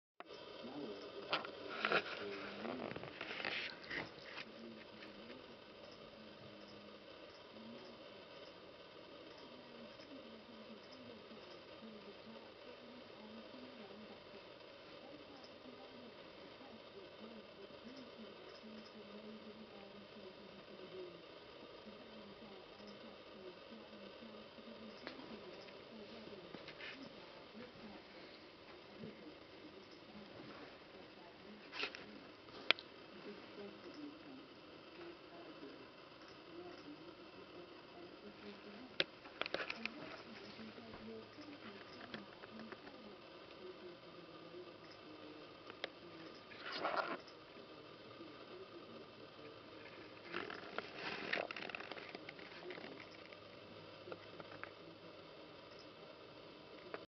sasha trying to sleep part 1
our cute cavalier king charles spaniel trying get some sleep.